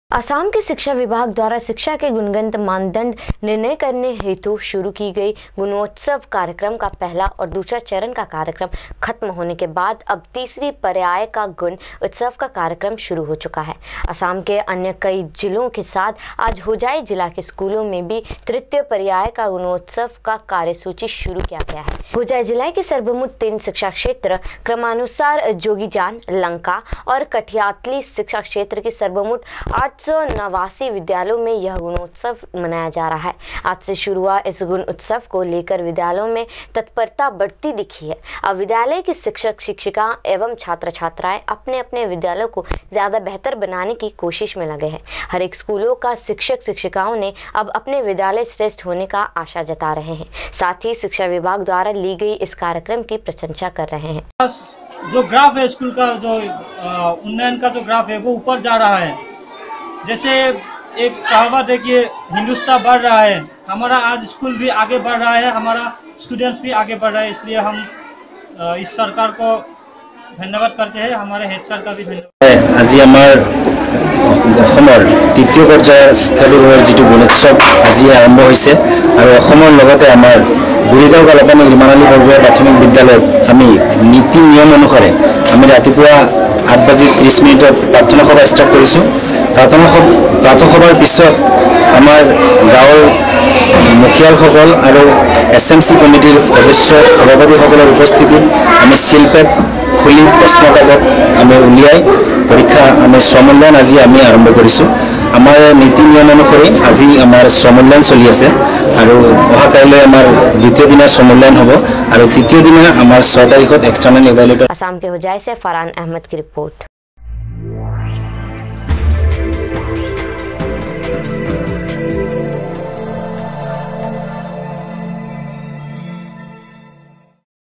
Assam News]Educational parameters of education were introduced by the education department of Assam
असम की शिक्षा विभाग द्वारा शिक्षा के गुणगत मानदंड निर्णय करे हेतु  शुरू की गई गुणोत्सव  कार्यक्रम का पहला और दूसरा चरण का कार्यक्रम खत्म होने के बाद अब तीसरी पर्याय का गुण उत्सव का कार्यक्रम शुरू हो चुका है